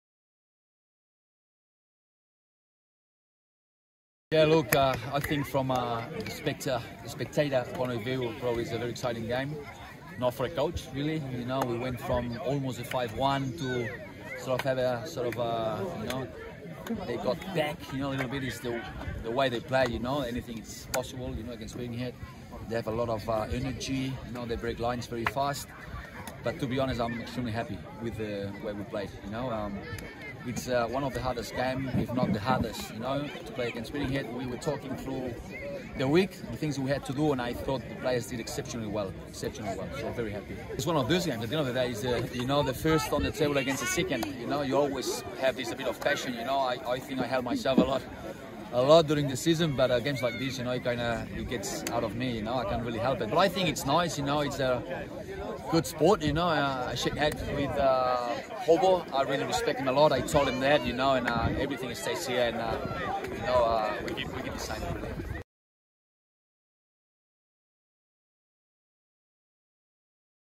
MATCH REACTION | Albert Riera, Birkenhead United, Wednesday 13 July 2022 | Northern League
Albert Riera gives his assessment of Auckland City FC's 5-3 away win over second placed Birkenhead United in the latest round of Northern League action played at Shepherds Park on Wednesday 13 July 2022.